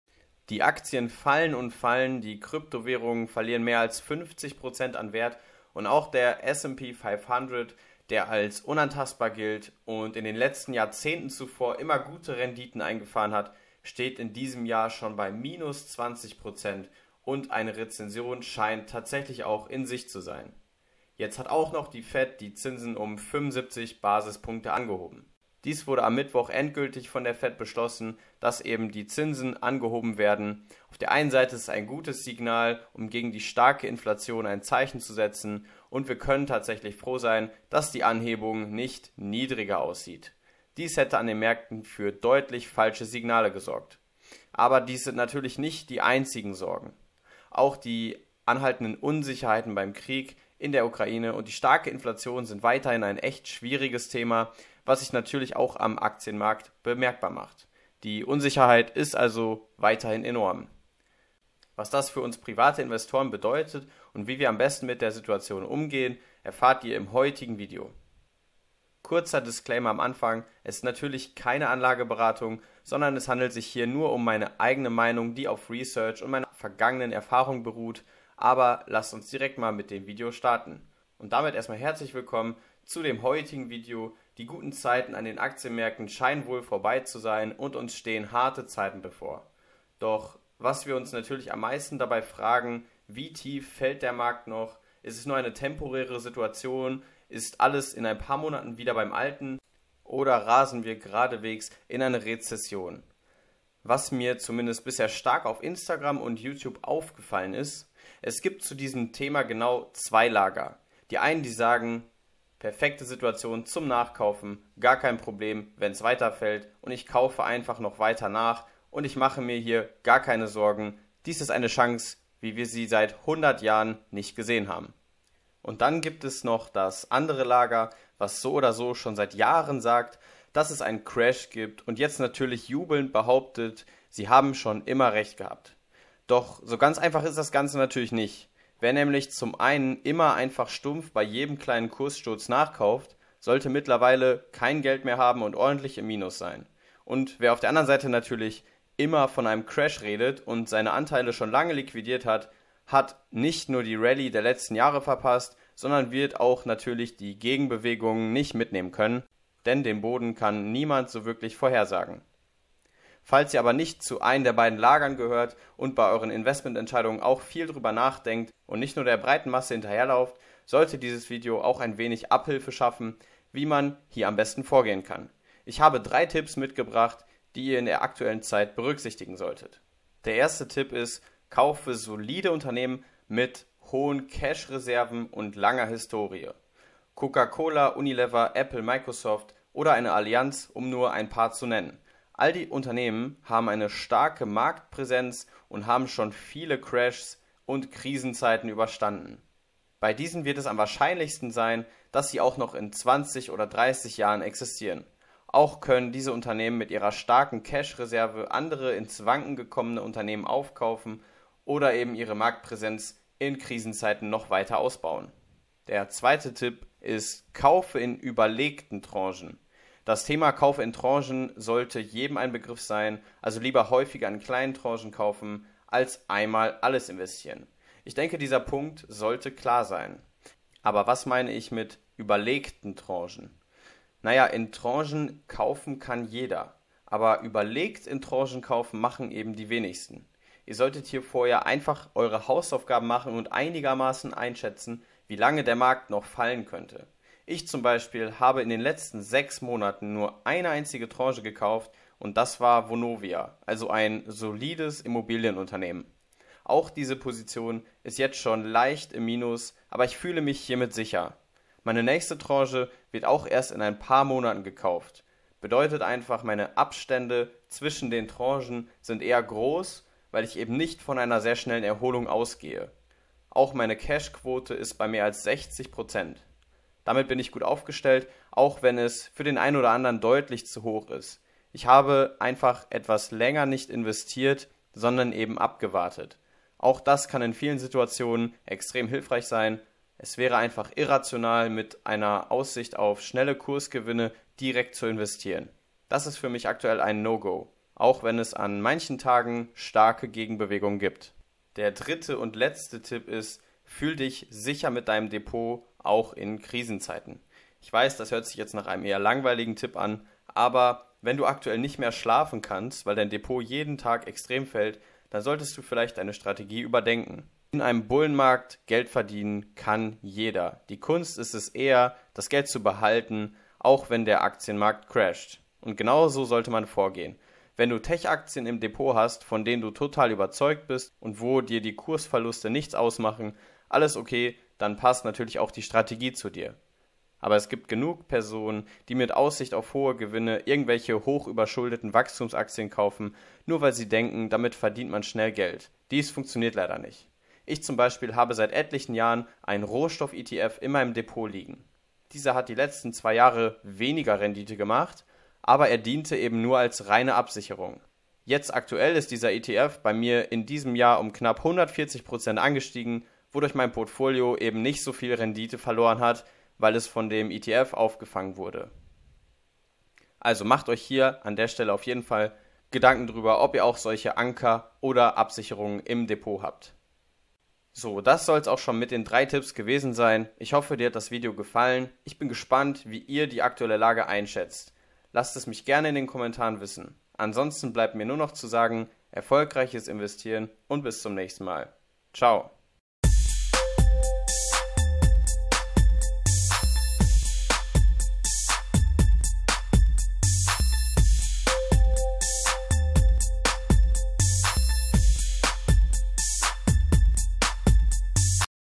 CHANCE des Jahrhunderts? So solltest du Vorgehen (Inflation, Rezession, günstige Aktien?)
Rezession, Inflation, Krieg, Zinsanhebungen. Diese 3 Tipps sollten dir dabei helfen, besser durch die schwierige Börsenzeit zu kommen.
Natürlich handelt es sich nur um meine eigene Meinung, ein Austausch ist wie immer erwünscht :) Um mich zu unterstützen wäre ich wie immer über einen Like + ein Abo sehr dankbar :)

Meine Depot-Empfehlungen:
Consorsbank (Sparpläne)*: